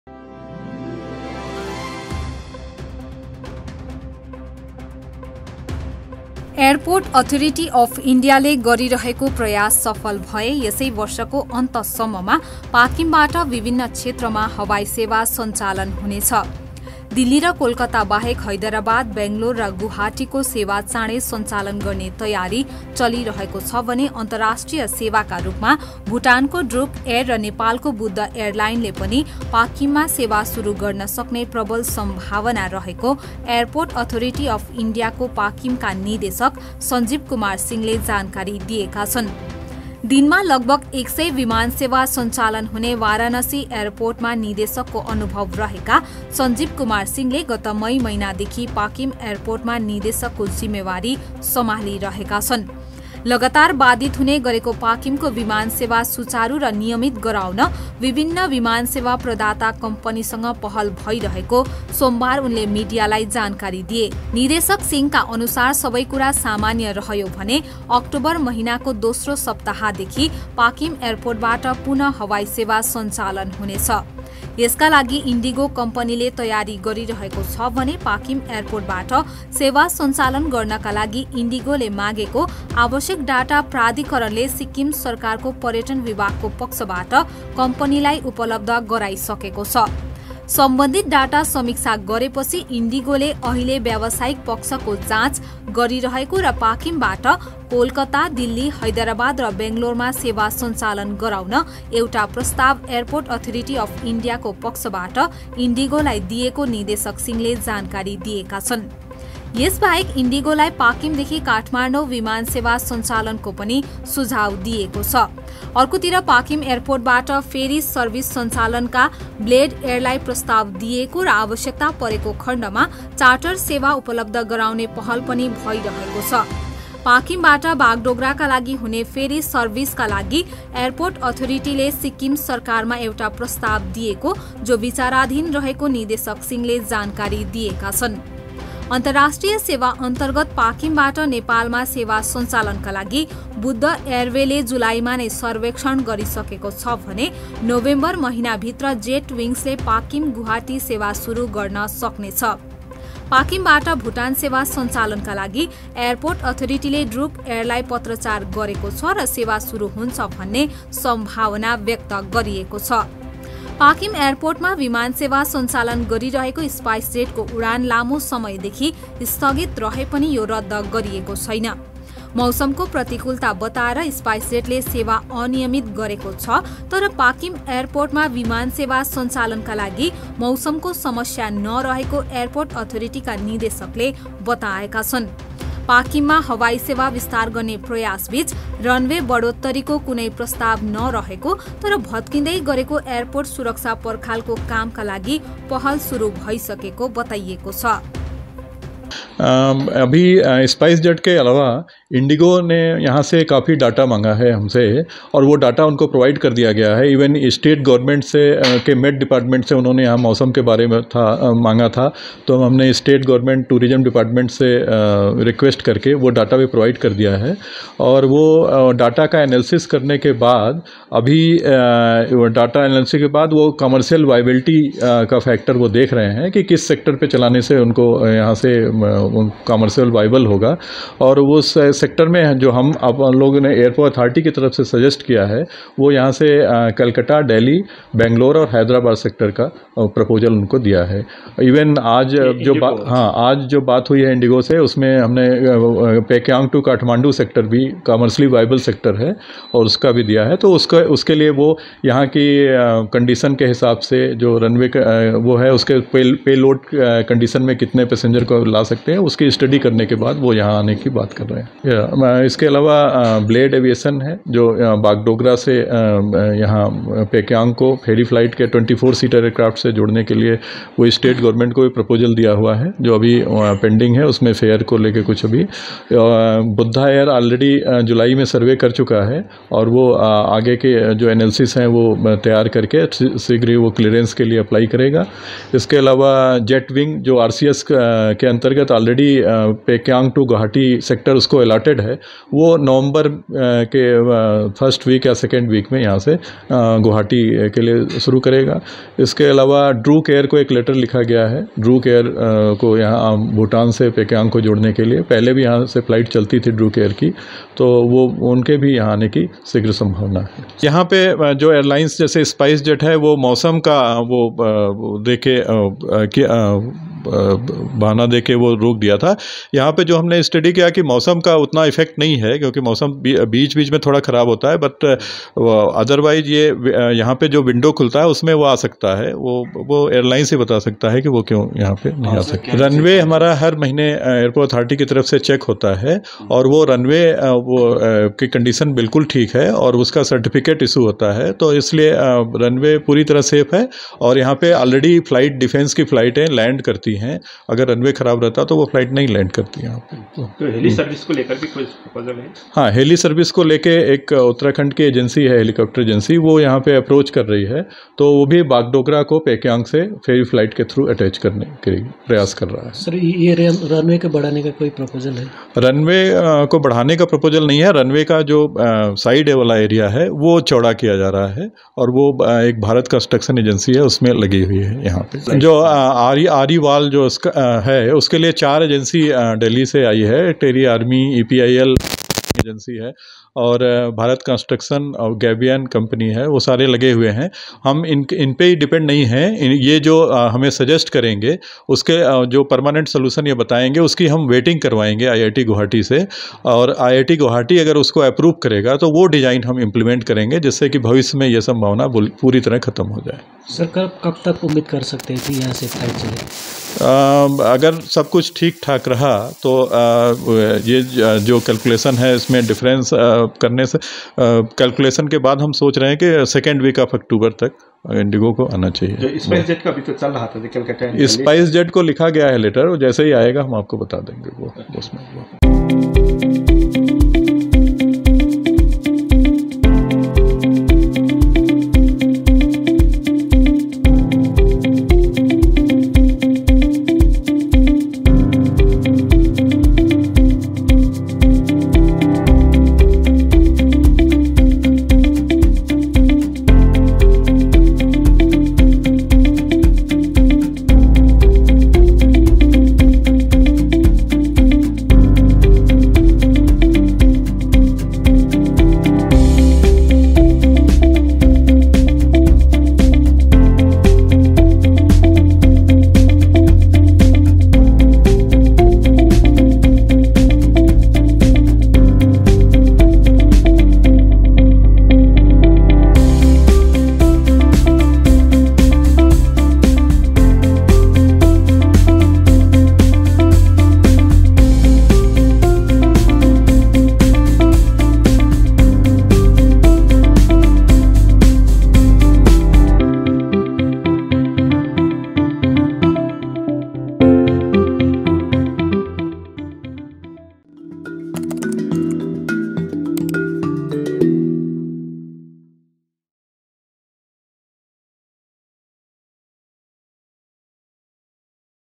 2024 –को अन्तसम्ममा पाकिमबाट हवाई सेवा सञ्चालन हुनसक्ने एयरपोर्ट अथोरिटीका निदेशकको भनाइ
2024 –को  अन्तसम्ममा पाकिमबाट विभिन्न क्षेत्रमा हवाई सेवा सञ्चालन हुनसक्ने एयरपोर्ट अथोरिटीका निदेशकको भनाइ